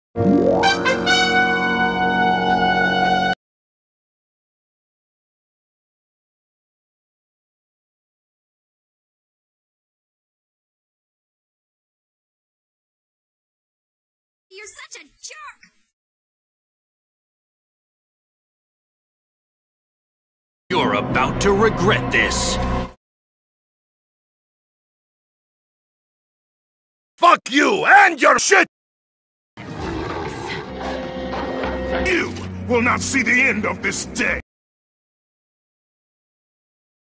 A Warning to Jake Rutigliano😡
This Jake dude is really mean to me and he's out of his mind he's typing rude comments on my videos and I don't like the way he typing stupid comment for bullying

Everyone block Jake Rutigliano he's insulting me!